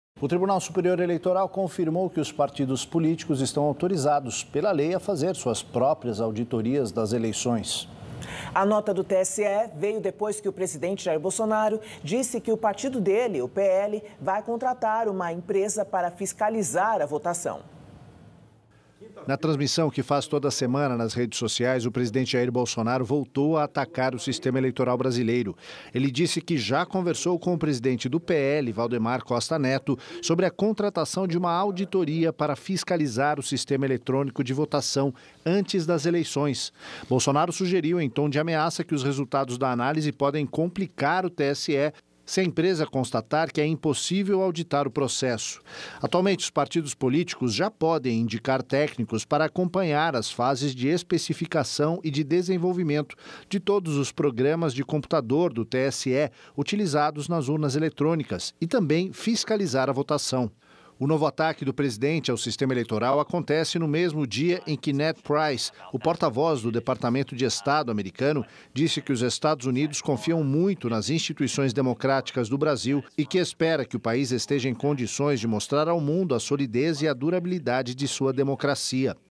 Após declaração de Bolsonaro, TSE afirma que partidos podem fazer auditorias privadas
Durante transmissão nas redes sociais, o presidente Jair Bolsonaro (PL) relatou que o seu partido irá contratar uma auditoria para fiscalizar o sistema eletrônico de votação para as eleições deste ano. A declaração surgiu como uma ameaça caso a empresa não fosse autorizada.

Em nota, o Tribunal Superior Eleitoral (TSE) confirmou que os partidos estão autorizados pela lei a fiscalizar a votação e podem indicar técnicos para acompanhar as fases de especificação e de desenvolvimento de todos os programas de computador utilizados nas urnas eletrônicas.